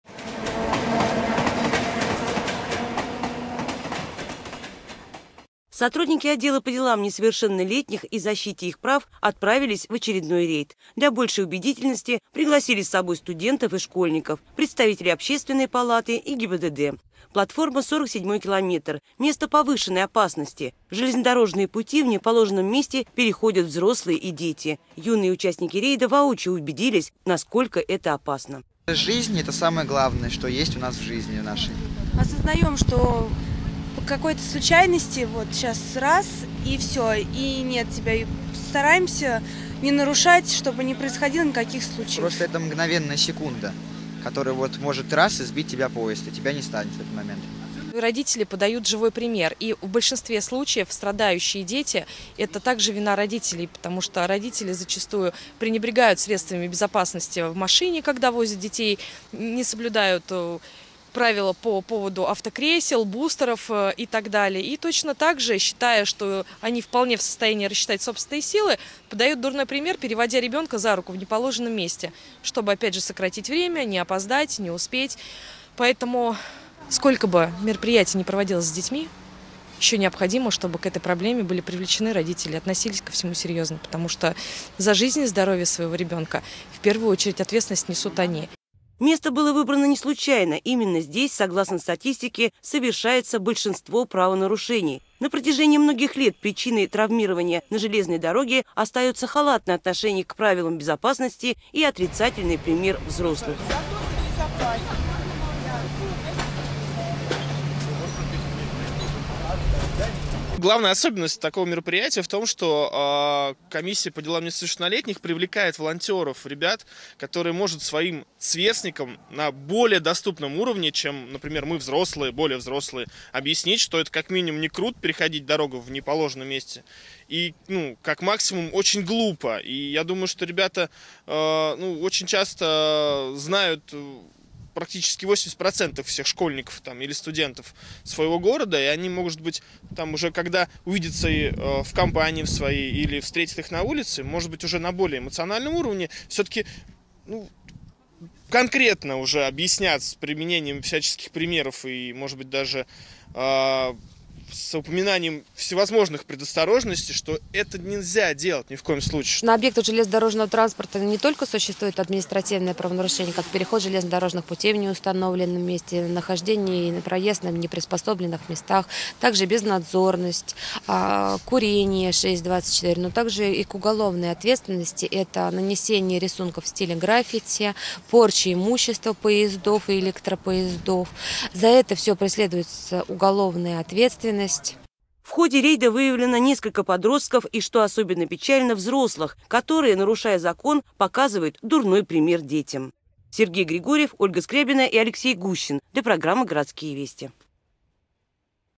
Рейд
Неоднократно сотрудники по делам несовершеннолетних  Раменского района выезжали в район платформы 47-ой километр. К сожалению, нарушителей по безопасному переходу железнодорожных путей меньше не становится. Было принято решение усилить это мероприятие и пригласить с собой в очередной рейд  детей.